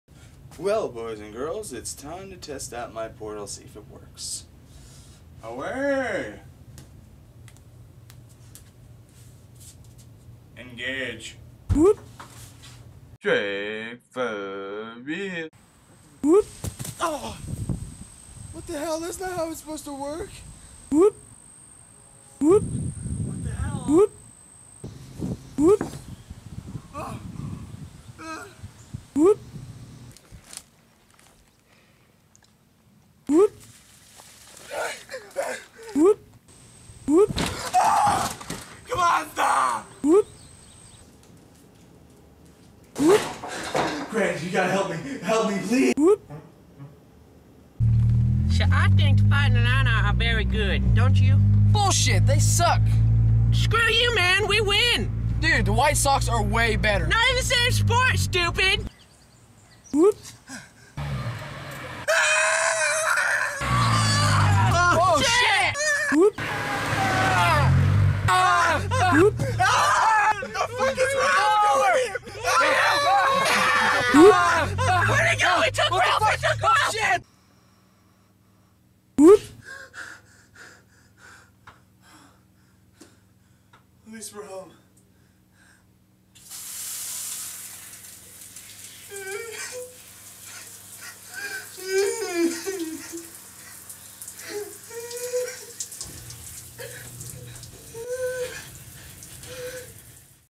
Transporter
I test out my new portal... It doesnt go so well..